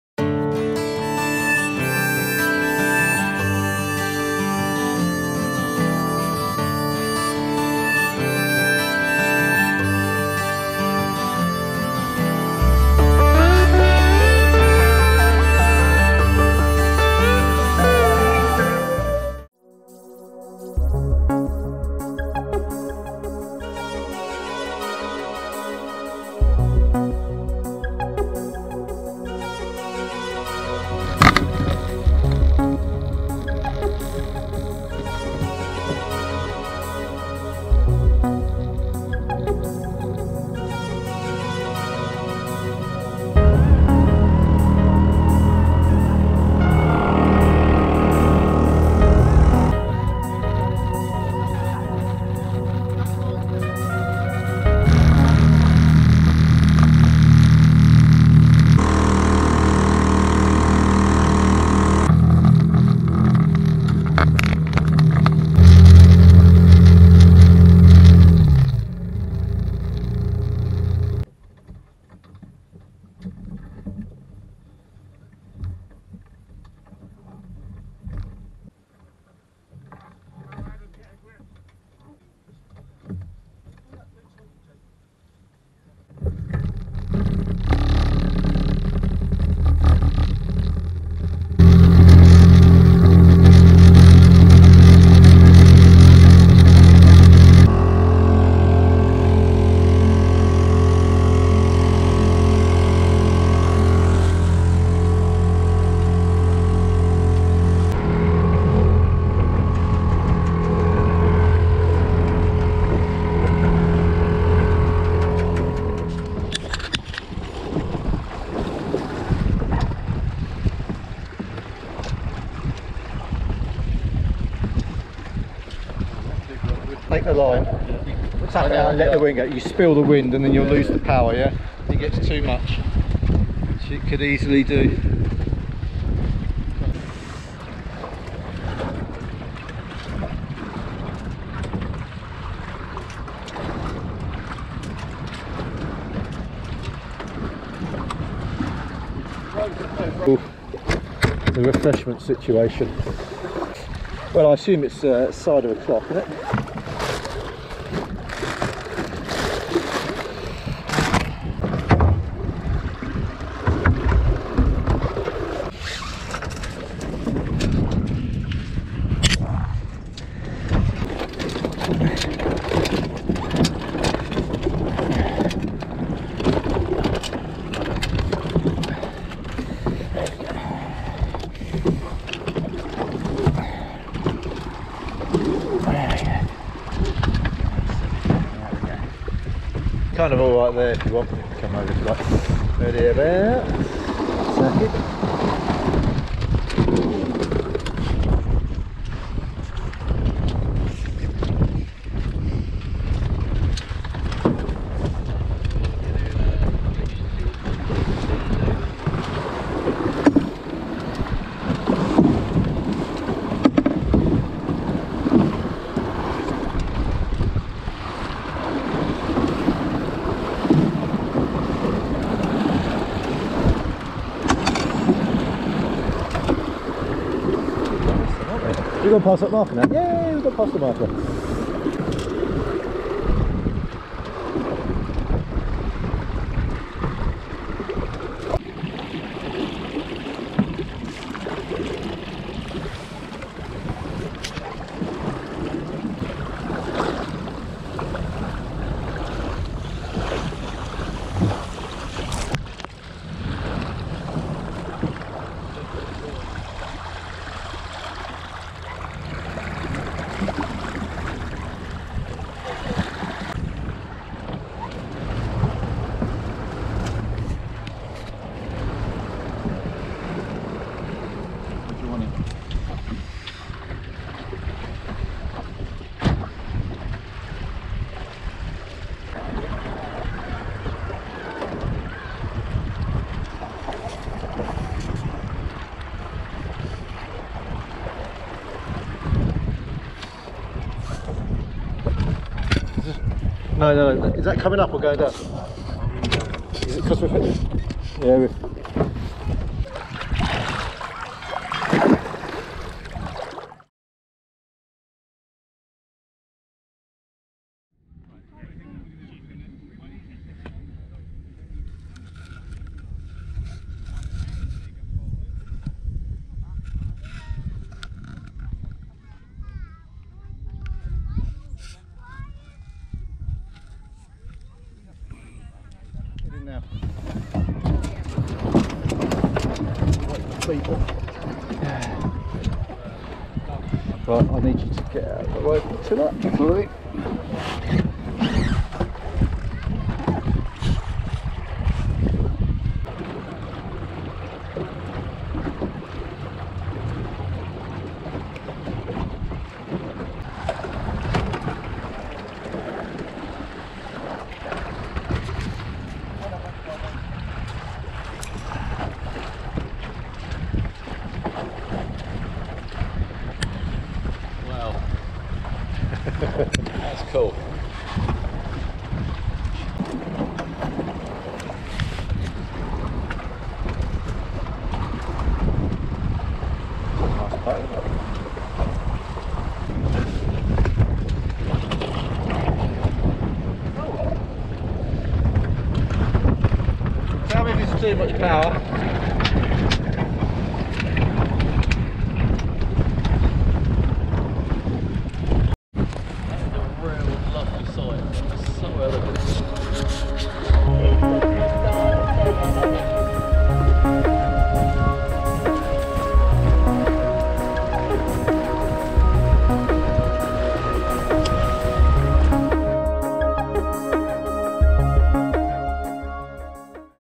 Solent & East Head Day Sail, Wanderer Dinghy Cruising Sailing Sail, Chichester Harbour
Sunny day motoring down the Harbour to Chichester Bar, Sailing along the South coast, lunch at East Head, Sailing over to Hayling Is before returning back.
Wanderer Dinghy Cruising Sailing Sail, Chichester Harbour, England UK